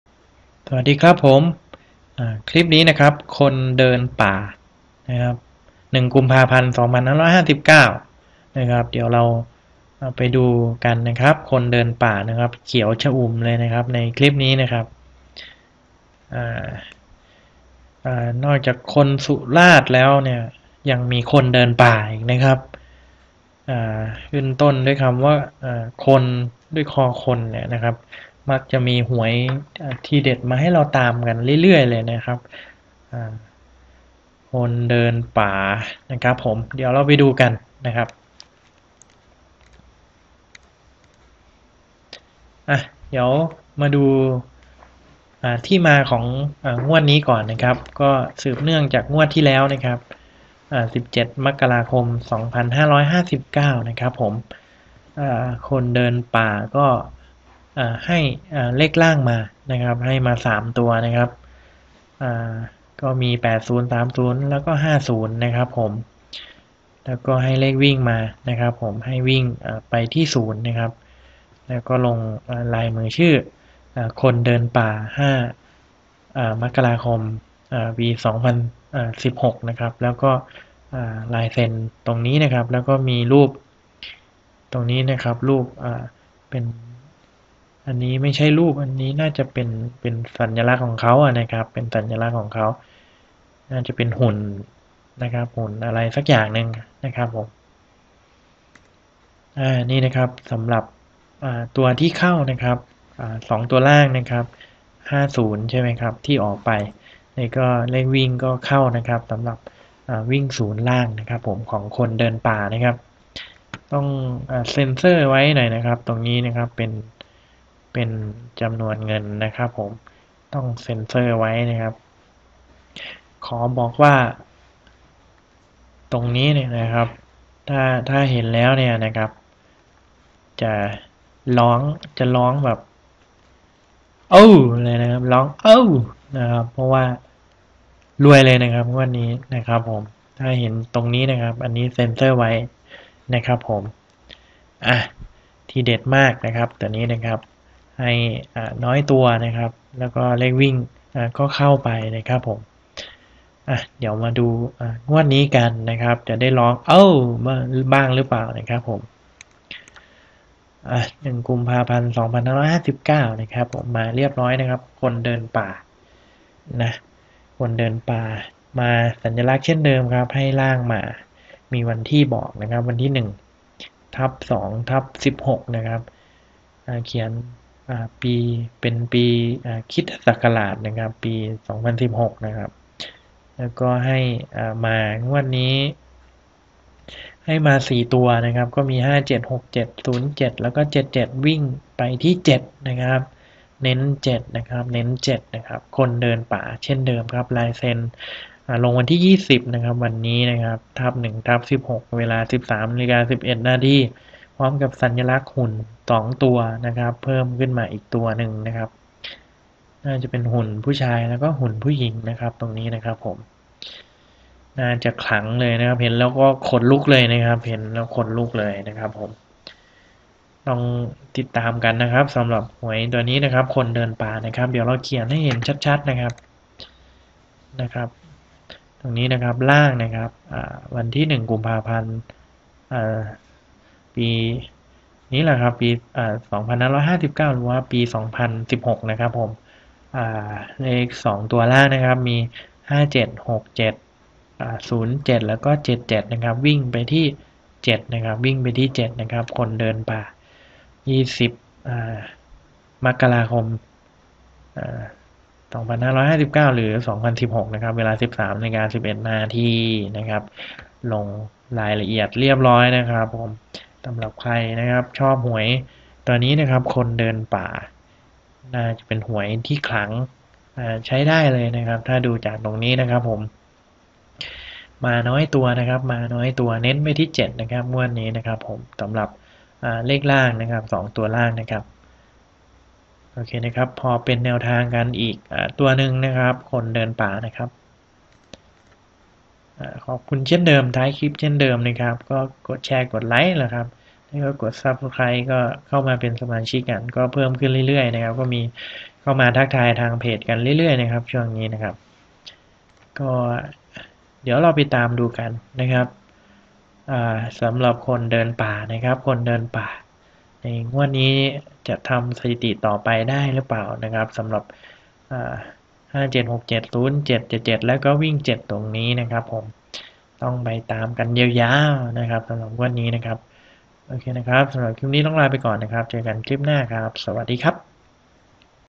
ฅนเดินป่า หวยทีเด็ด 2 ตัวล่าง งวดที่แล้วได้ร้องโอ้วกันไปแล้ว งวดนี้ 1/2/59 จะได้ร้องโอ้วอีกหรือไม่
หวยของ "ฅนเดินป่า" นับเป็นหวยทีเด็ดมาก เป็นการให้หวย  2 ตัวล่างหรือเลขท้าย 2 ตัว ซึ่งในงวดที่แล้วได้ร้องโอ้ว ดีใจกันไปแล้วกับการเข้ามหาศาล และงวดนี้ 1/2/59 เลข 2 ตัวล่างมาพร้อมกับเลขวิ่งทีเด็ดเรียบร้อยแล้ว จะได้ร้องโอ้วฉลองกันอีกหรือไม่ ตามกันครับ(ใช้เป็นแนวทางในการซื้อสลากกินแบ่งรัฐบาลบาลเท่านั้น โปรดใช้วิจารณญาณในการรับชม)